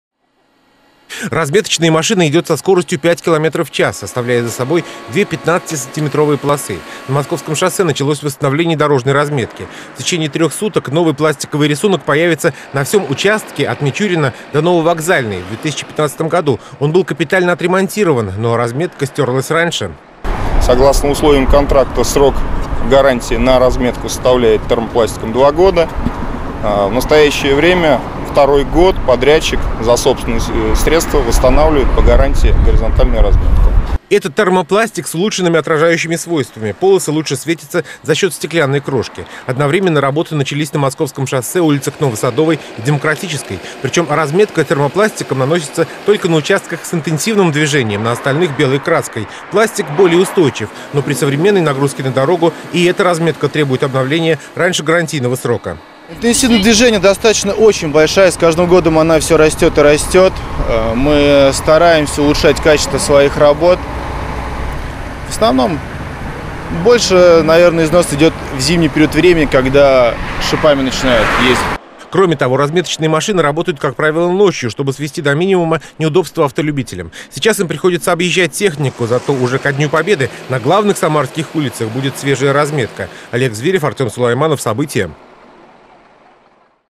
На Московском шоссе восстанавливают дорожную разметку
Свежая разметка ко Дню Победы. На Московском шоссе начались работы по восстановлению маркировочного рисунка. Техника выходит ночью, чтобы не создавать пробки. Сейчас полосы термопластиком наносят на гарантийном участке.